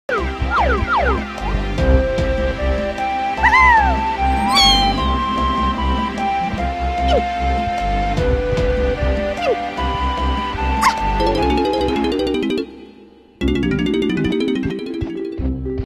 Crystal Caves Lobby Early With Tiny
I think this was found by Unreal a while back. The only other video I know of was deleted, so I made another.